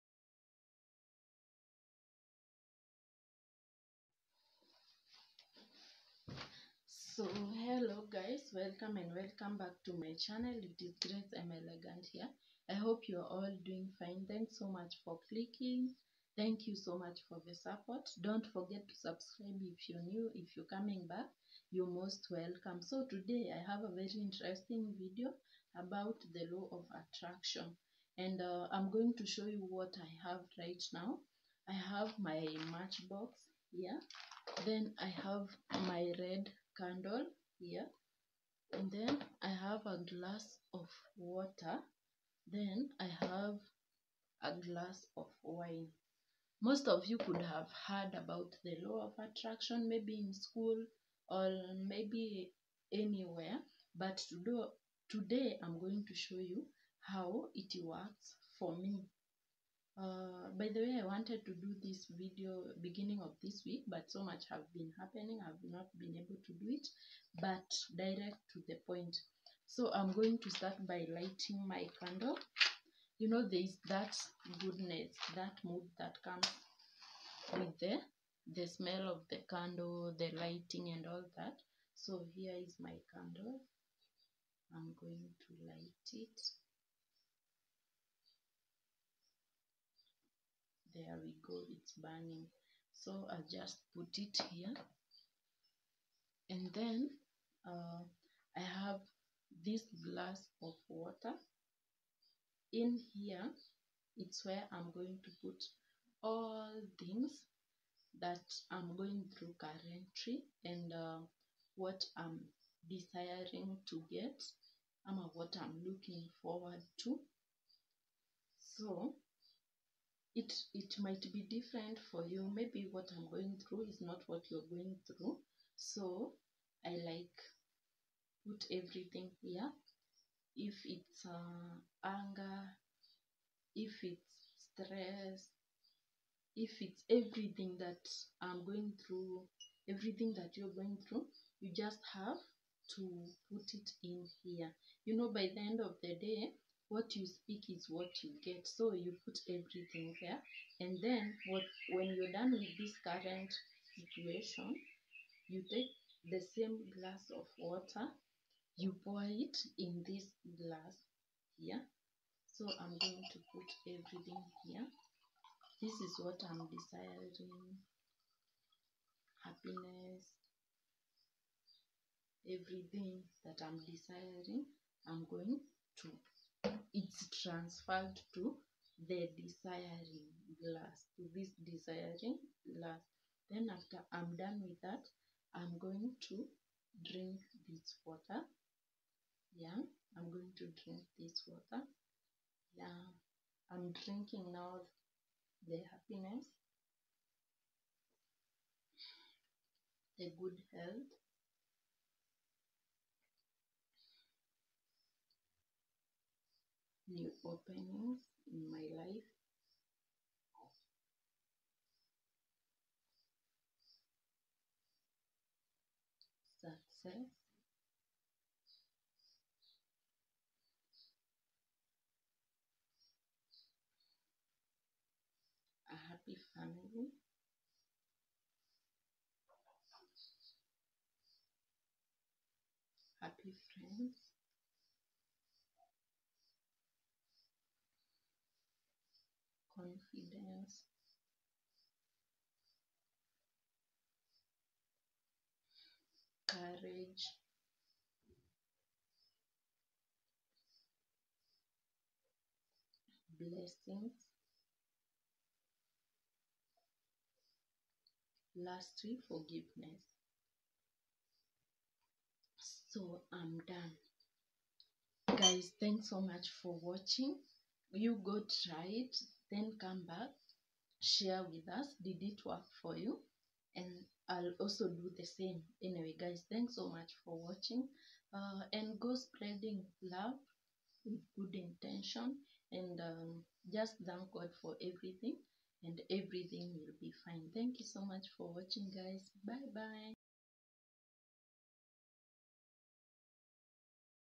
vibe#lawofattraction#magic#overnightmagic How I applied the law of Attraction
In the video I used 2 glasses ,lighter,water and a candle to do the magic.